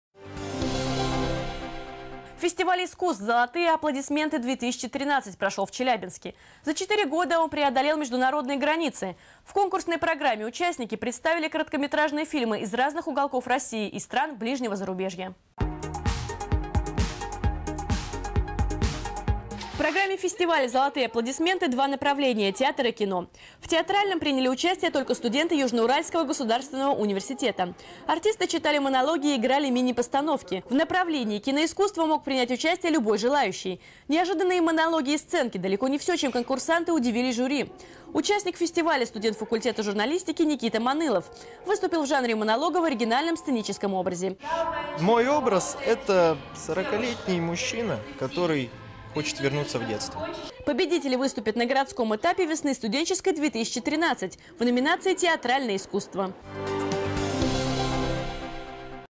Фестиваль искусств «Золотые аплодисменты-2013»
За 4 года он преодолел международные границы. В программе фестиваля «Золотые аплодисменты» два направления: театр и кино. В театральном -  приняли участие только студенты Южно-Уральского государственного университета. В направлении кино-искусство мог принять участие любой желающий. Артисты читали монологи и играли мини-постановки. Победители выступят на городском этапе «Весны студенческой-2013» в номинации «Театральное искусство».